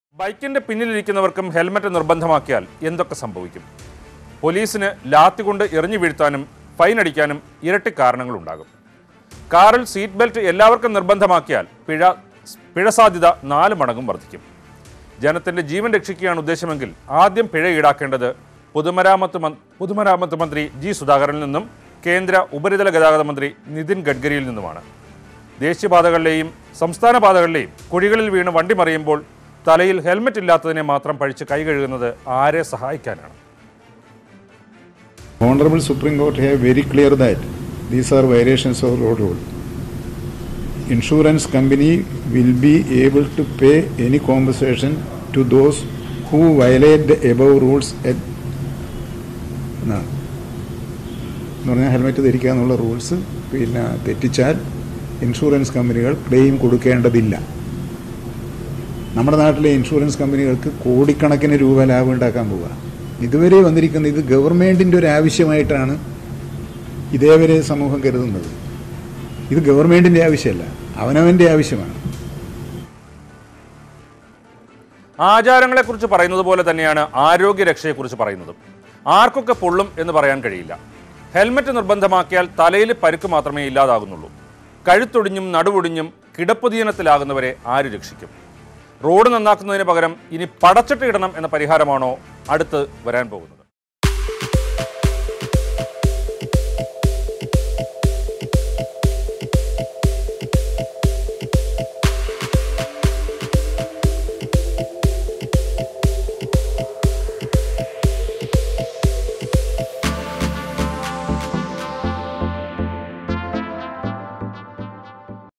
പിന്‍സീറ്റ് യാത്രക്കാര്‍ക്ക് സീറ്റ് ബെല്‍റ്റും ഹെല്‍മറ്റും നിര്‍ബന്ധമാക്കിയാല്‍ ഉറപ്പാകുമോ സുരക്ഷ?
The State Transport Department has asked the State Transport Commissioner and Police chief to make ensure that the pillion riders in the state wears the helmet at all times. The government has also made the seat belt for back seat passengers in four wheelers mandatory.